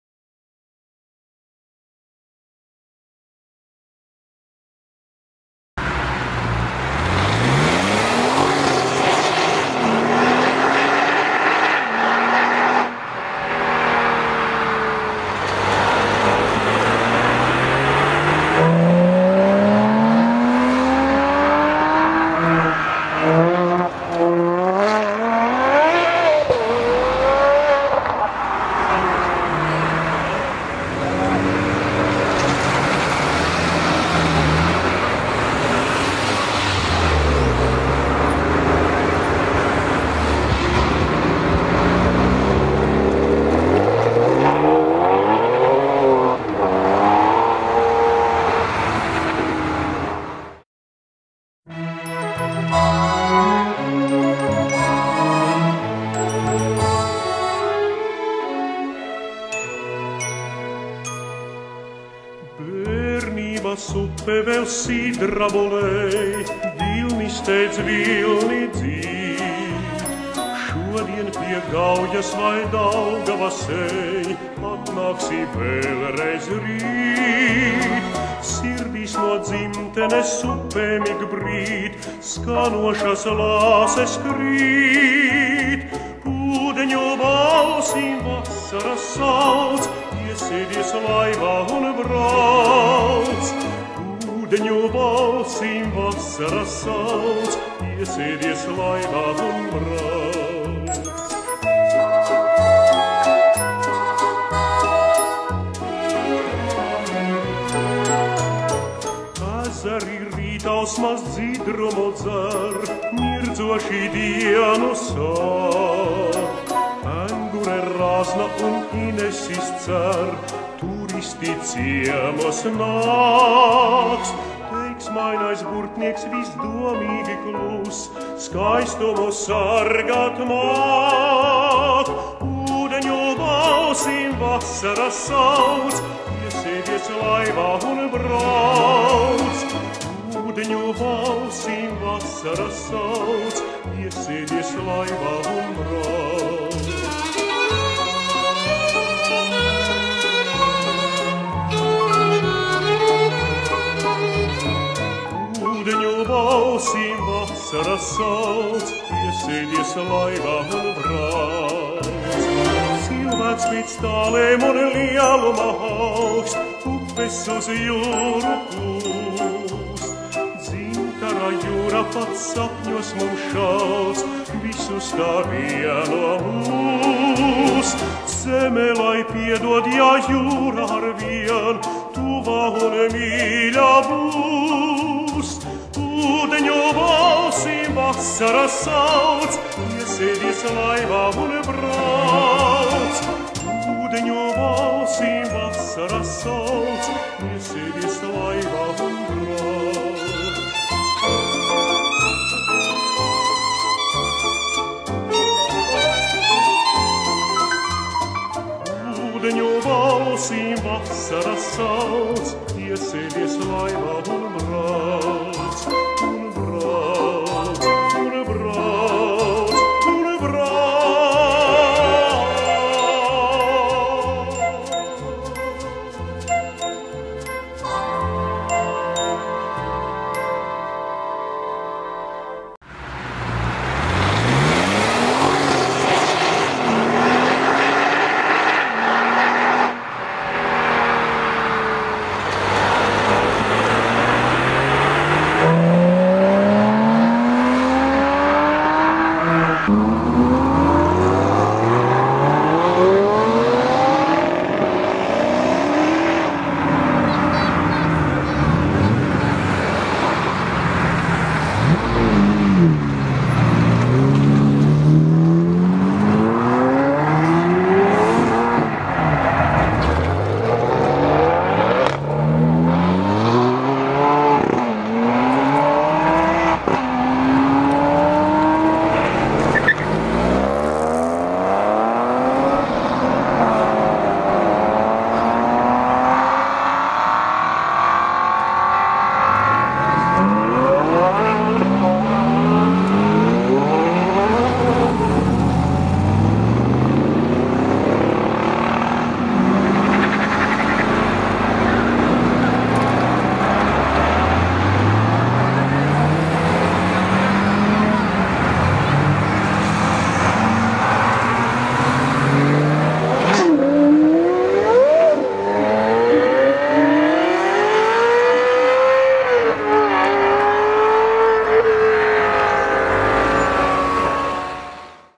Ūdeņi sauc 2014
Vasaras izskaņas laivošana pa Ineša ezerā :)